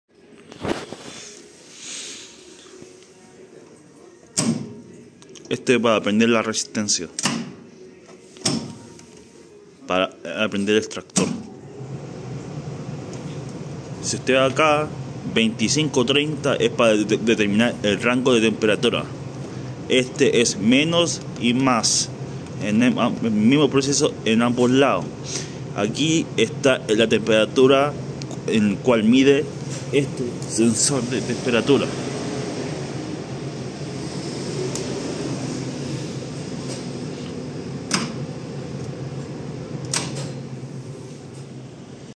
Extractor calefactor electrico industrial trifasico 10KW instruccion
Ficha Técnica
Potencia nominal:     10 KW
Voltaje:     380V
Volumen de aire:     3000
Area de calefacción:     60-100
Motor del ventilador:     .37KW
Amperio:     17.5A